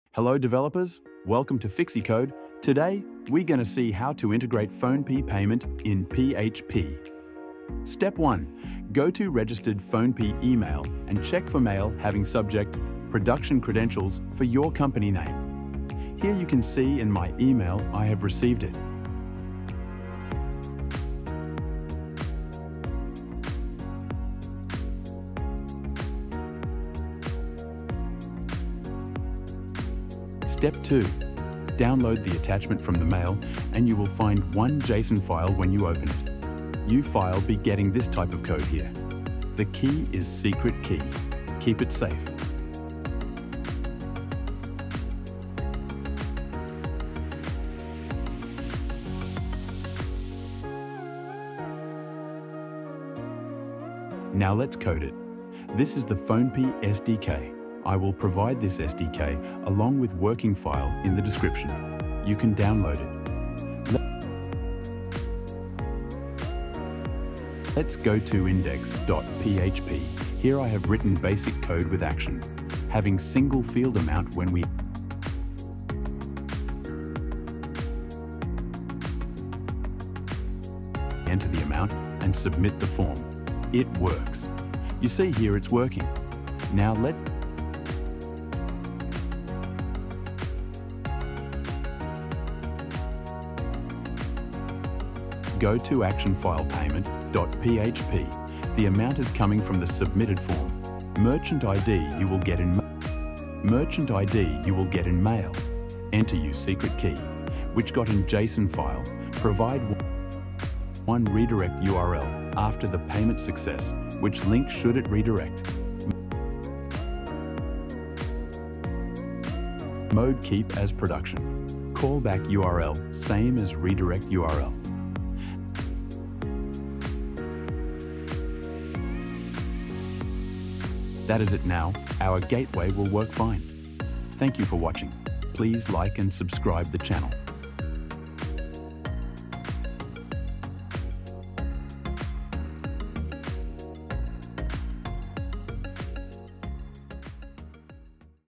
PHP PhonePe Payment Gateway Integration | Step-by-Step Guide with Working Example
In this tutorial, we will walk you through the process of integrating the PhonePe payment gateway into your PHP project. Whether you're building an e-commerce site or any other application that requires online payments, this step-by-step guide will help you set up and implement PhonePe as a secure and efficient payment option.

🔧 What You'll Learn:

Introduction to PhonePe payment gateway
Setting up your PhonePe merchant account
Generating API keys for integration
Writing the PHP code to process payments
Handling callbacks and responses
Testing the integration with live and sandbox environments
📁 Resources:

Download the working example code [Link to your code]
Official PhonePe API documentation [Link to documentation]